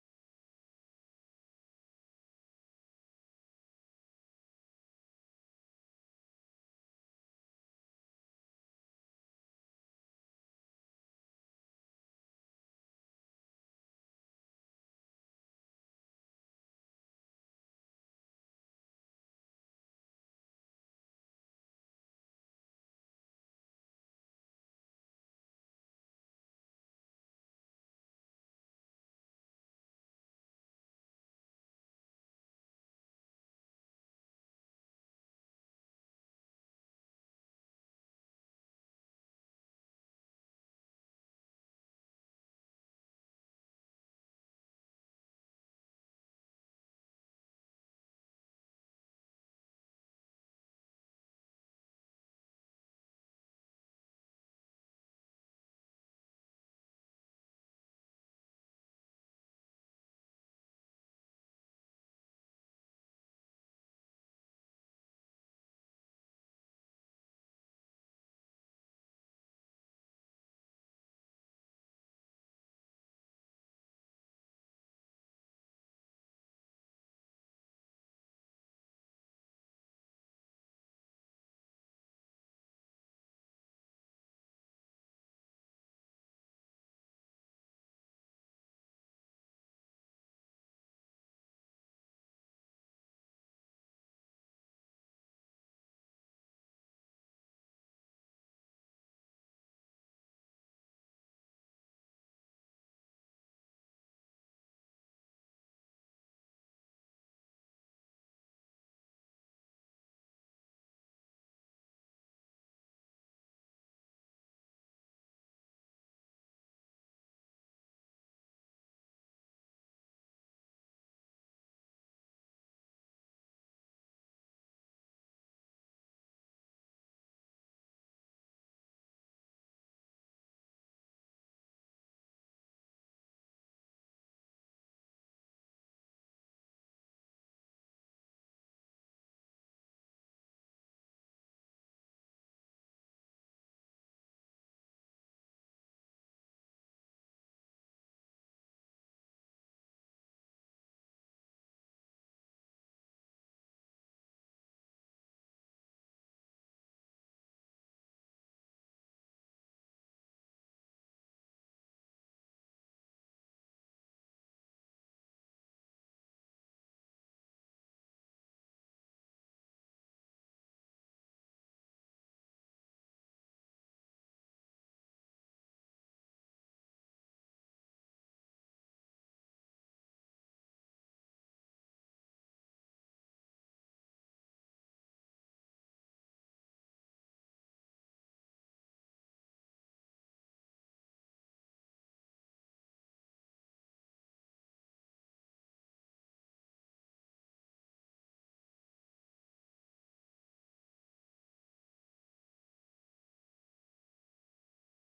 Fox Photo Highlights June 2021 9
Ninth compilation of fox photos from June 2021.
Redone with animated slides.